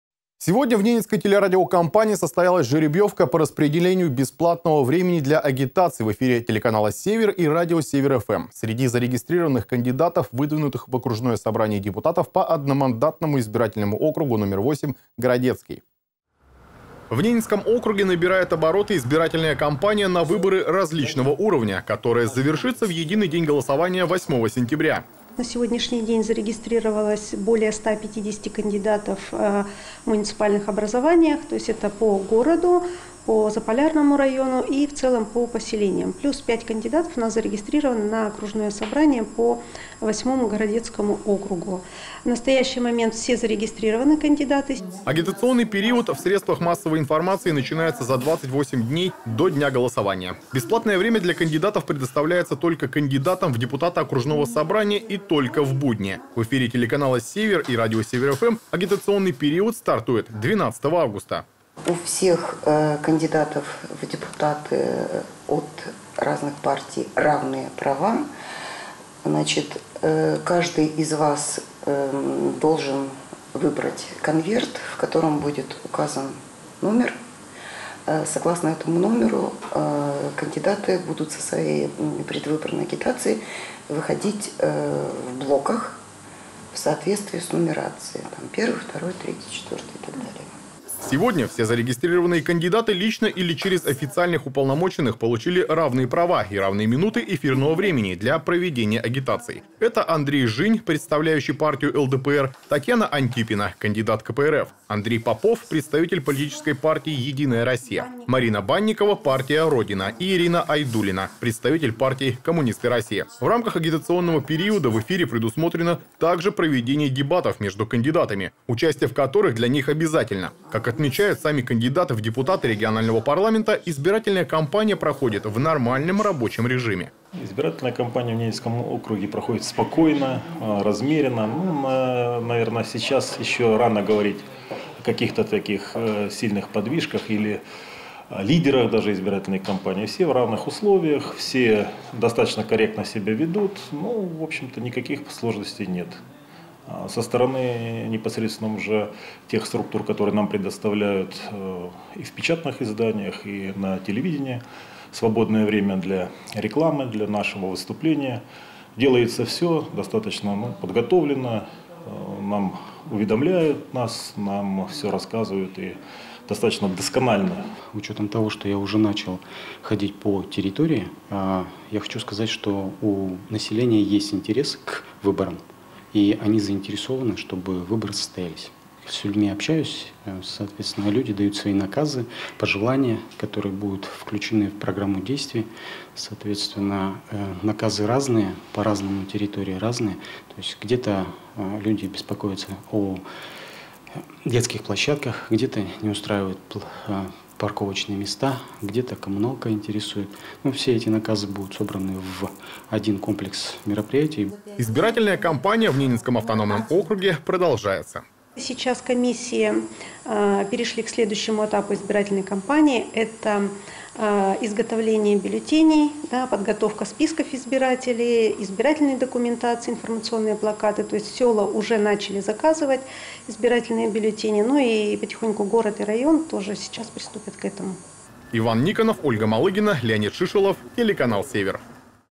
ИЗБИРАТЕЛЬНАЯ КАМПАНИЯ 2019
Сегодня в Ненецкой телерадиокомпании состоялась жеребьевка по распределению бесплатного времени для агитации в эфире телеканала Север и радио Север ФМ среди зарегистрированных кандидатов, выдвинутых в окружное Собрание депутатов по одномандатному избирательному округу № 8 Городецкий.